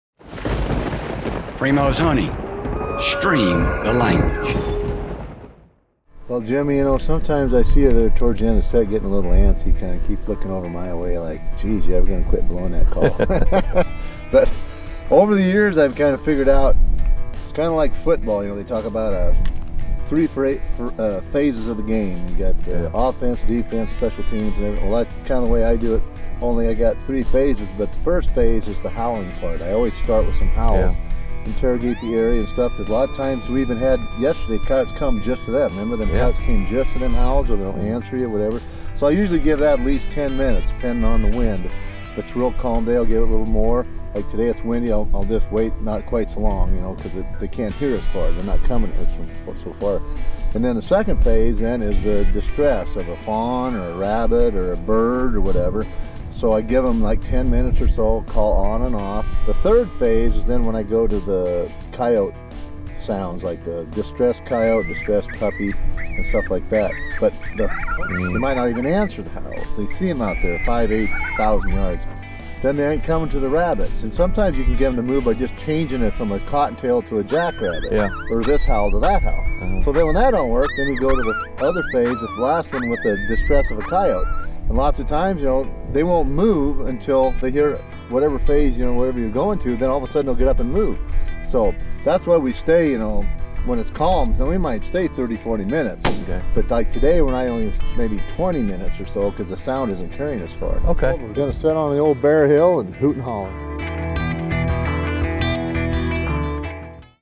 Randy Anderson's 3 Stages of Calling in Coyotes
Predator Master Randy Anderson explains his 3 Stages of Calling in Coyotes.

Save on Primos hunting gear right now!

Personalize a great gift at Primos Custom Mill Shop!